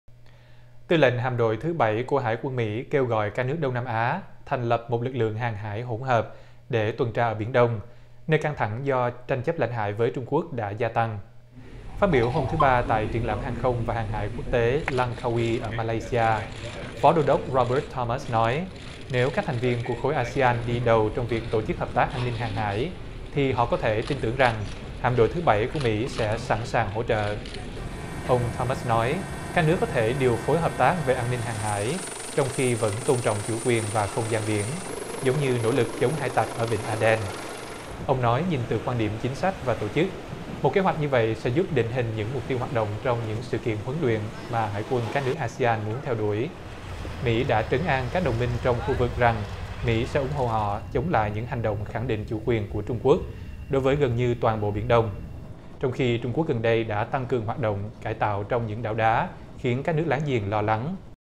Hải quân Mỹ kêu gọi ASEAN lập lực lượng tuần tra hỗn hợp ở Biển Đông
Tin tức Nếu không vào được VOA, xin các bạn hãy vào để vượt tường lửa. Tư lệnh Hạm đội thứ Bảy của Hải quân Mỹ kêu gọi các nước Đông Nam Á thành lập một lực lượng hàng hải hỗn hợp để tuần tra ở Biển Đông, nơi căng thẳng do tranh chấp lãnh hải với Trung Quốc đã gia tăng. Phát biểu hôm thứ Ba tại Triển lãm Hàng không và Hàng hải Quốc tế Langkawi ở Malaysia, Phó Đô đốc Robert Thomas nói nếu các thành viên của khối ASEAN đi đầu trong việc tổ chức hợp tác an ninh hàng hải thì họ có thể tin tưởng rằng Hạm đội thứ Bảy của Mỹ sẽ sẵn sàng hỗ trợ.